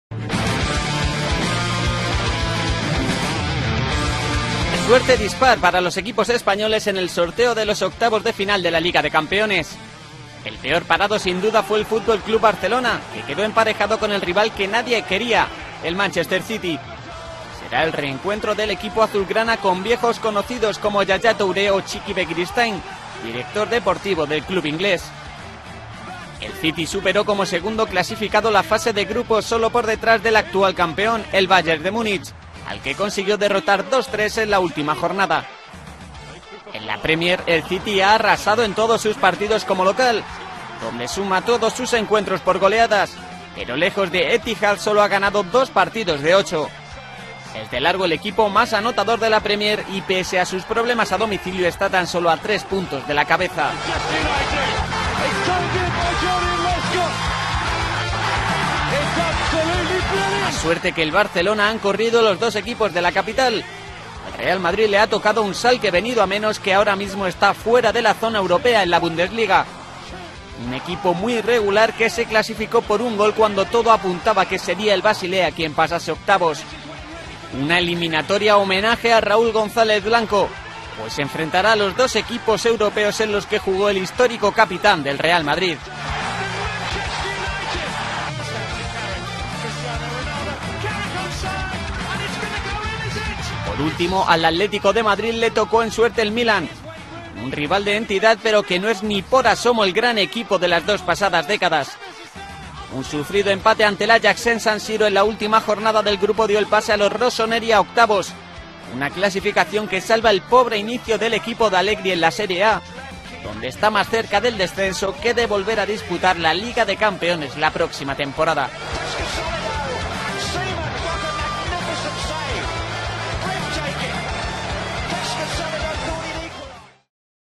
champions league 2013-14 draw of 16 teams and their contributions with talents
VERY TOUGH DRAW FOR THE CATALANS, WHO MEASURED THE WORST POSSIBLE OPPONENT IN THE SECOND ROUND, AND MORE AFFORDABLE FOR ATLÉTICO MADRID.Bombing of the draw for the knockout stages of the Champions League. Barca will face Manchester City, the most feared opponent . More fortunate were the Madrid, who face Schalke 04 , and Atletico, which will intersect with Milan .